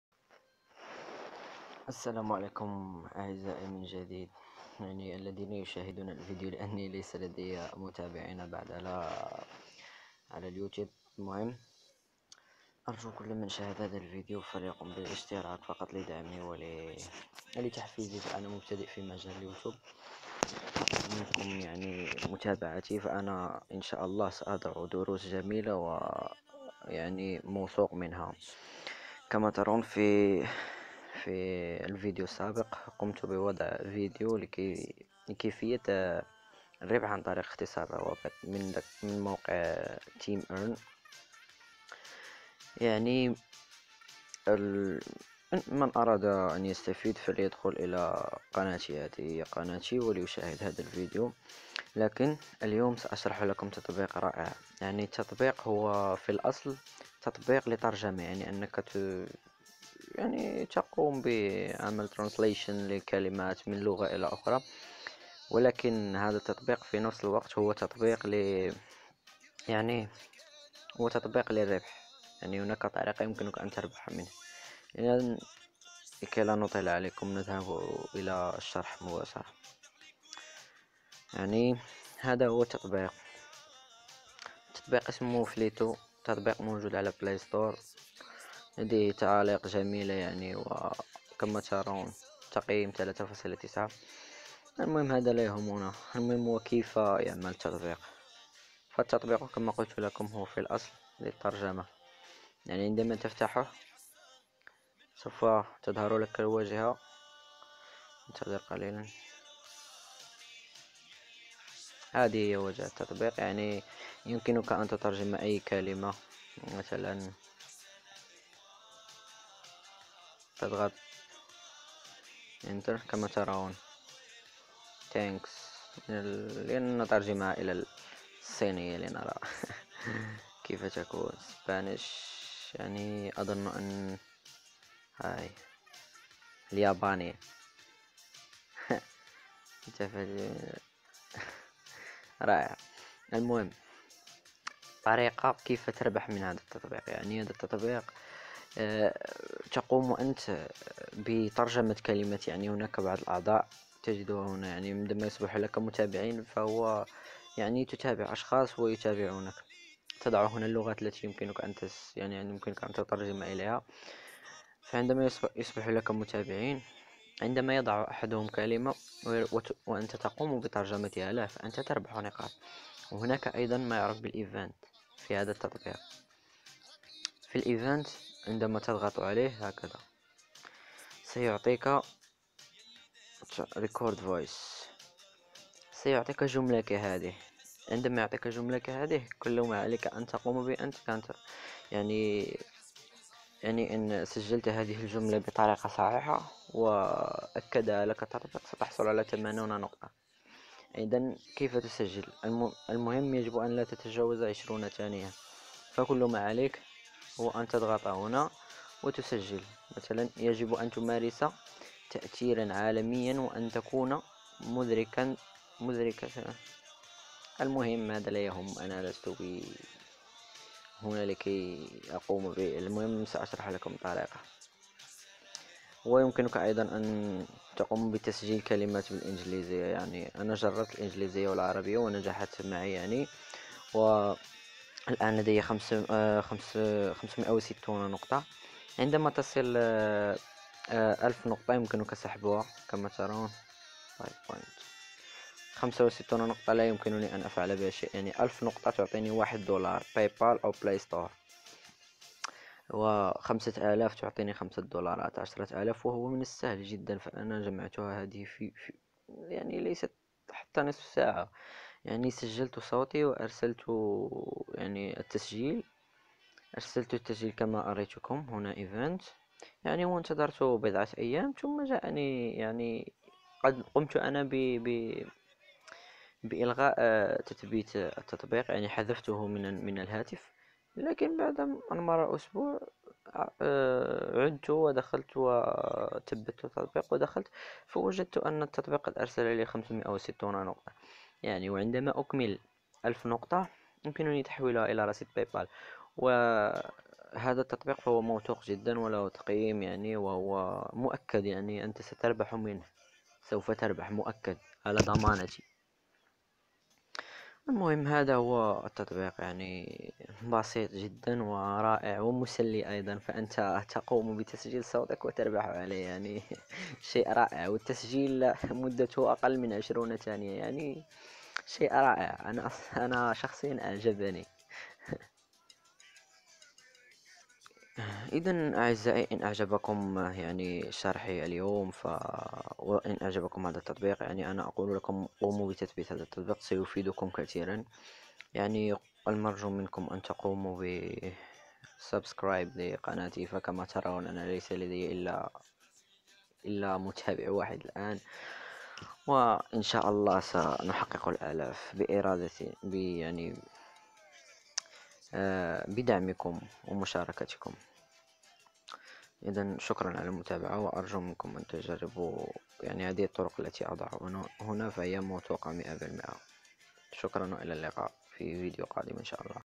الطريقة #2 للربح من الانترنت تطبيق flitto تطبيق خرافي اربح من تسجيل صوتك و الترجمة
التطبيق ستجده في بلاي ستوور فقط ابحت عن flitto